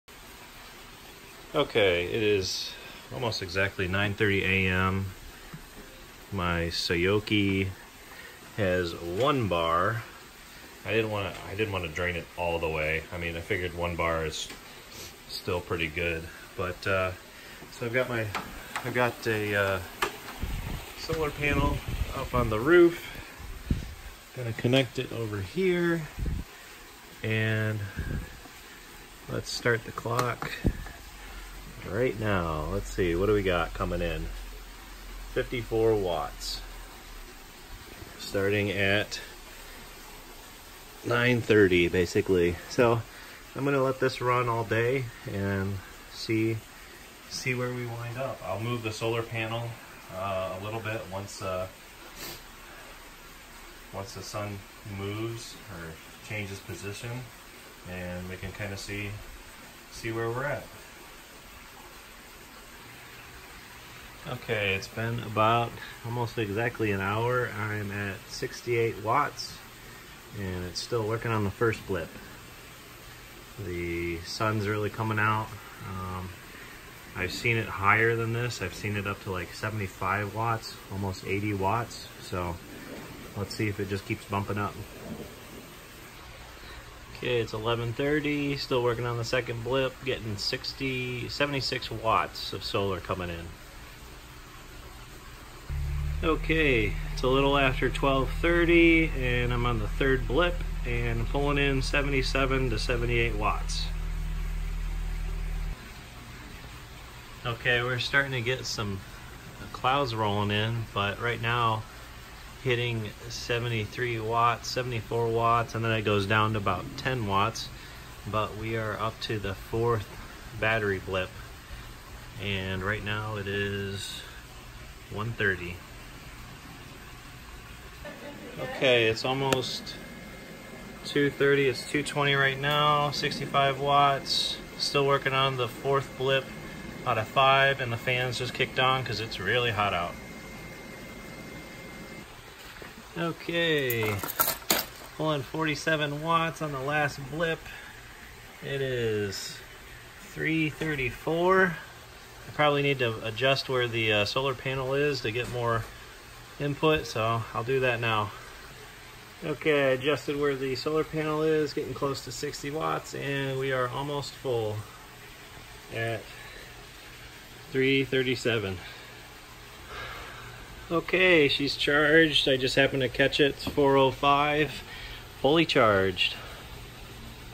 Testing how long it takes to charge the Suaoki G500 from 1 bar to 5 bars with solar
Testing the G500 solar charging with one 100 watt flexible solar panel. It took 6.5 hours to go from 1 bar to full.